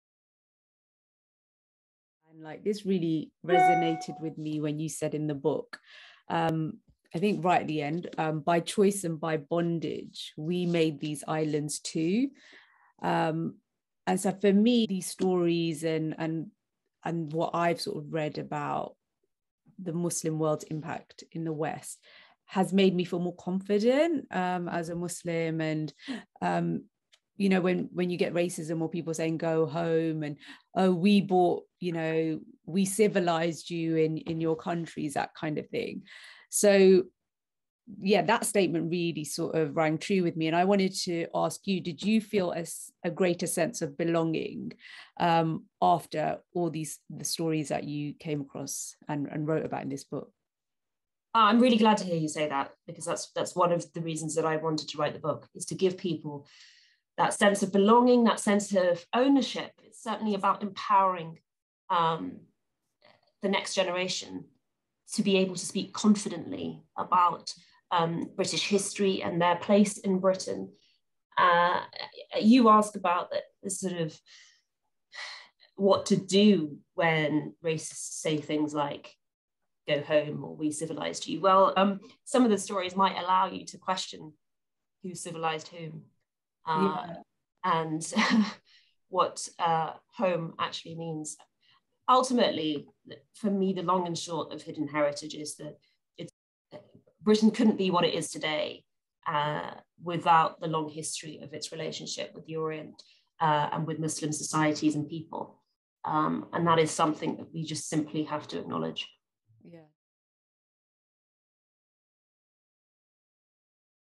Interview with Fatima Manji: Author of Hidden Heritage (EXTRACT)
For Islamophobia Awareness Month, I talk to Fatima Manji, Channel 4 News presenter and Author about her new book 'Hidden Heritage. It is a fascinating, eye opening book which tells the story of Britain long-standing connection with the Muslim world